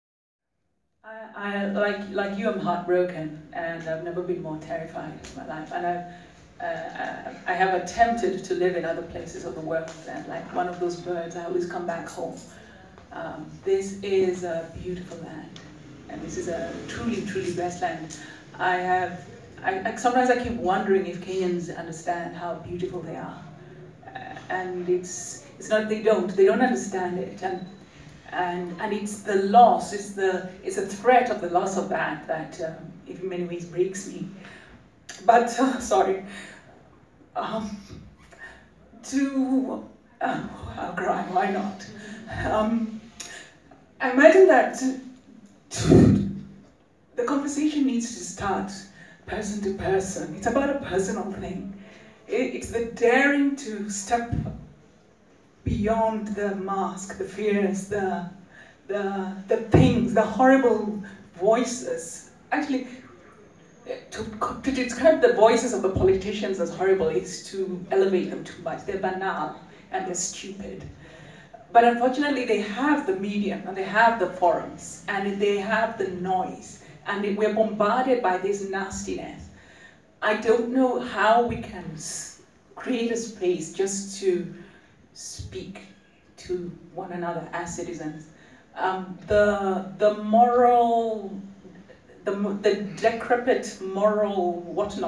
A tearful Yvonne Adhiambo Owuor speaks
A tearful Yvonne Adhiambo Owuor speaks at the Goethe on 25th May 2016.